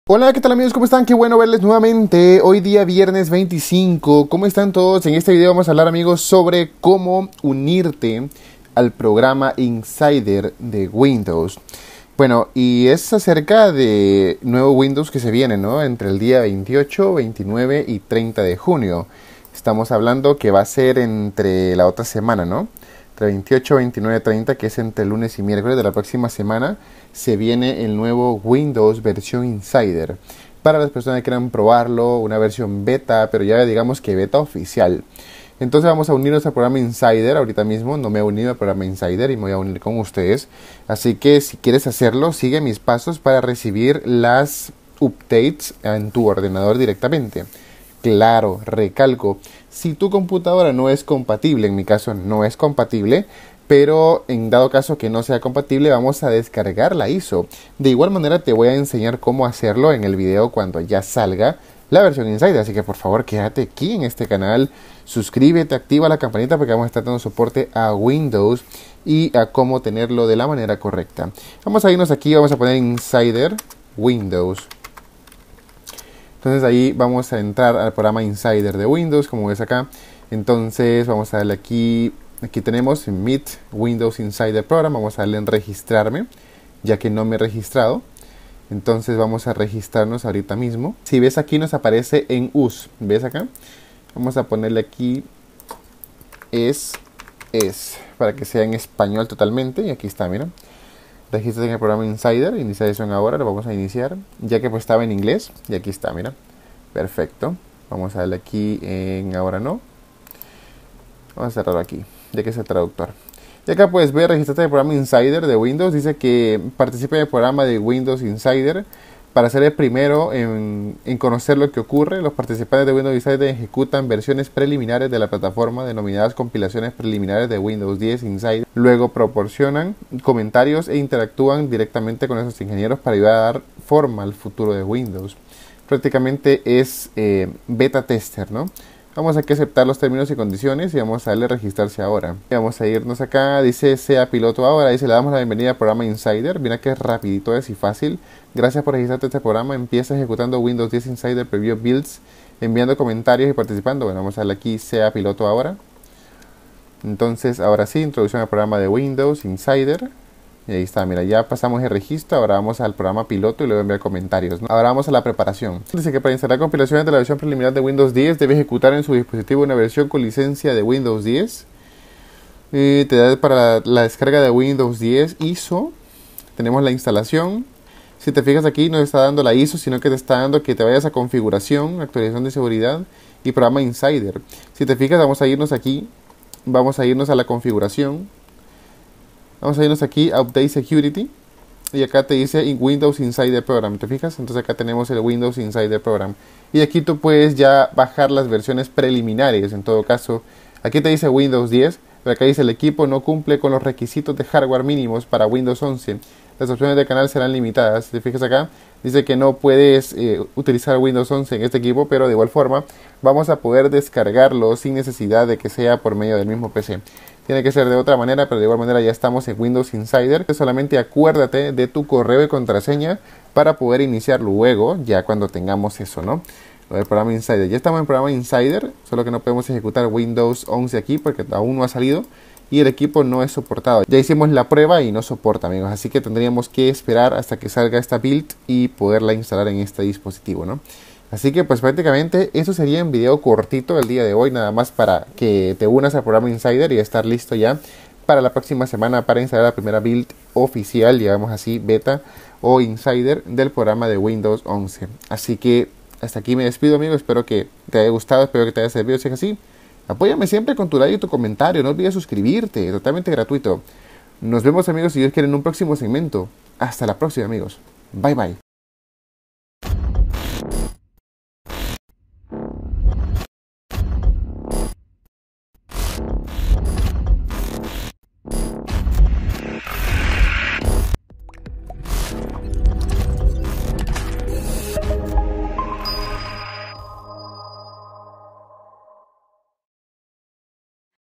✅ Como Unirte Programa Insider De Windows 11 En PC NO SOPORTADAS - Pruébalo el 28 de junio😎
APOYAME CON TU SUSCRIPCIÓN Y COMPARTIENDO

TEAM:

LOS ILUSIONISTAS DE LA TECNOLOGÍA.!

🌕Junio 2021🌕